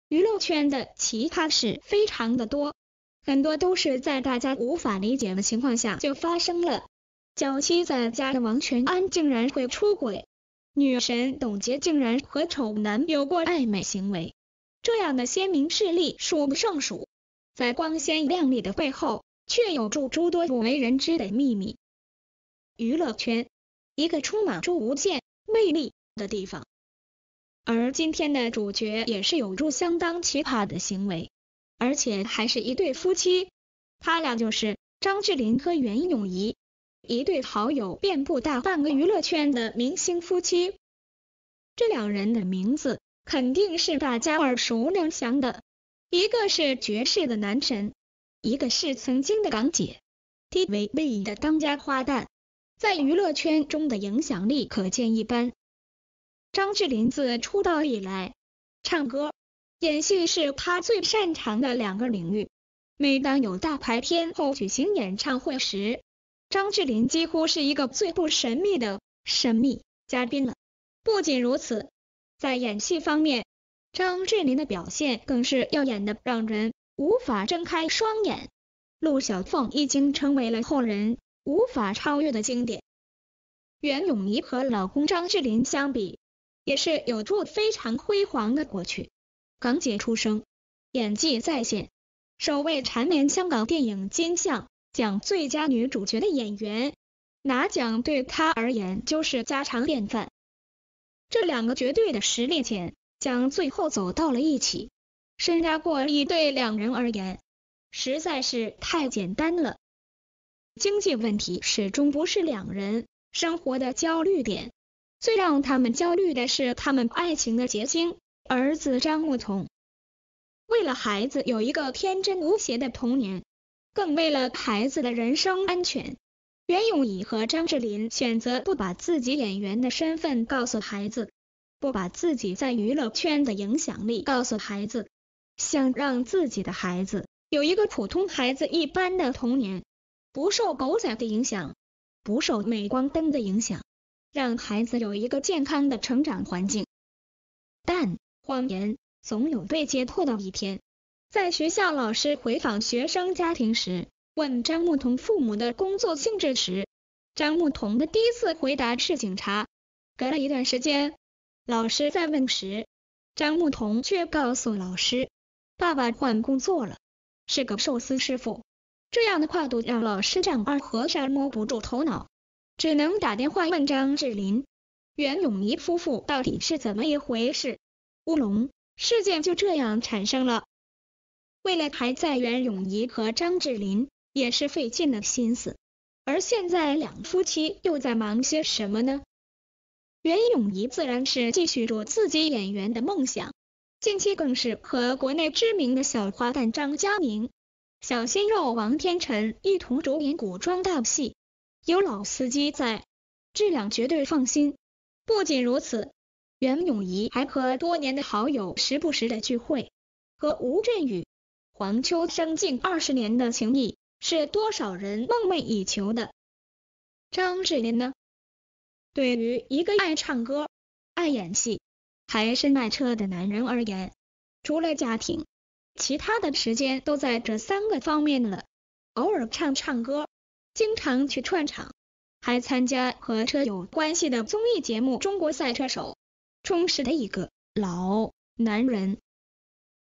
夫妻兩人身家過億，紅遍娛樂圈，卻不敢告訴兒子這一切，孩子一直以為爸爸是警察！ ！
視頻簡介：夫妻兩人身家過億，紅遍娛樂圈，卻不敢告訴兒子這一切，孩子一直以為爸爸是警察！ ！